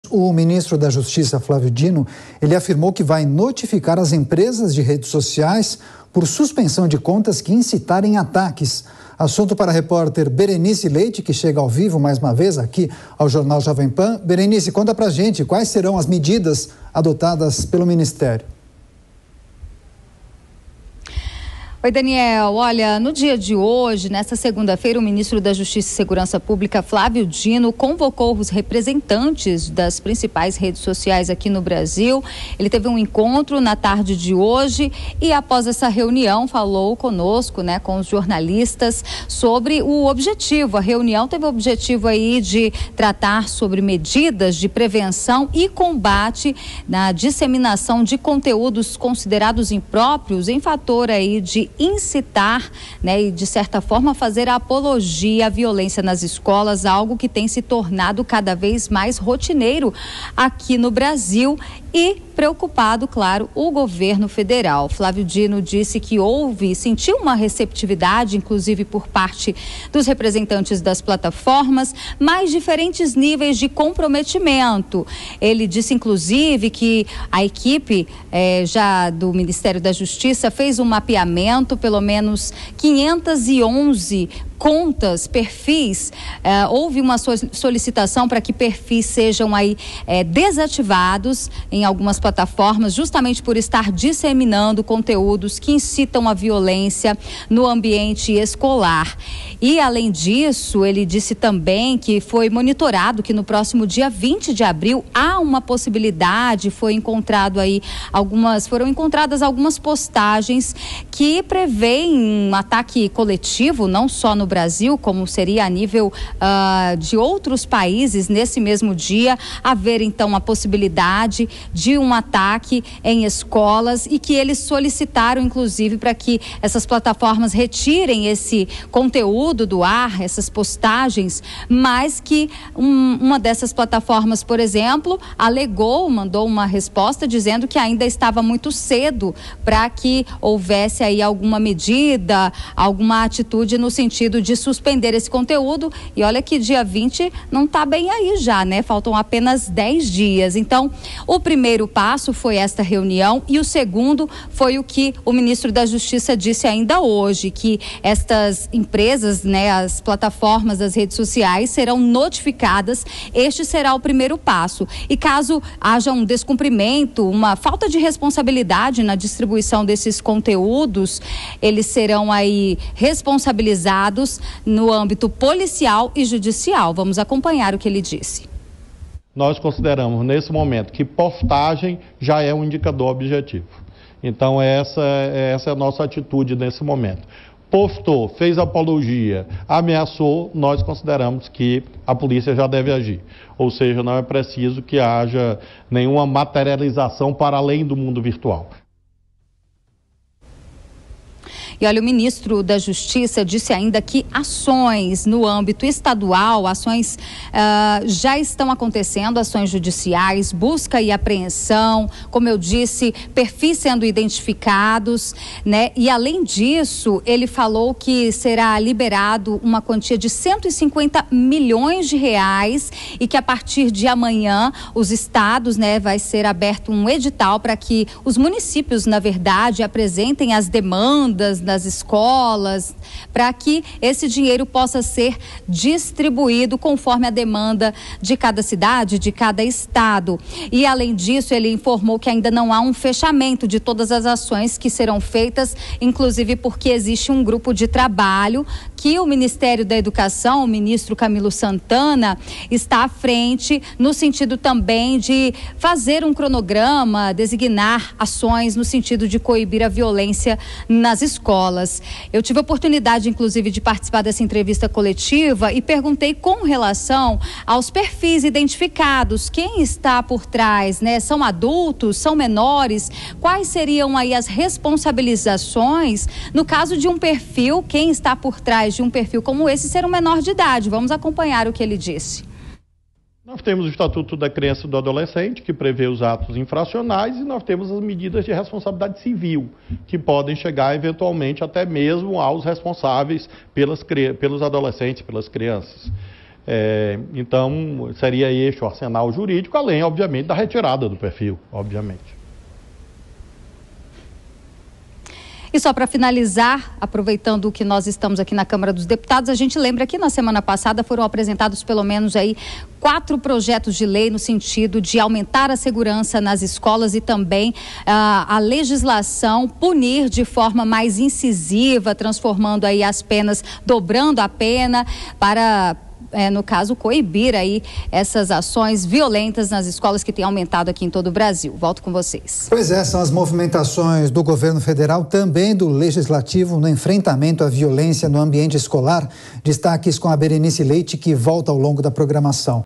Flávio Dino vai notificar redes sociais por suspensão de perfis suspeitos
Flávio Dino afirmou que já apresentou 511 pedidos ao Twitter.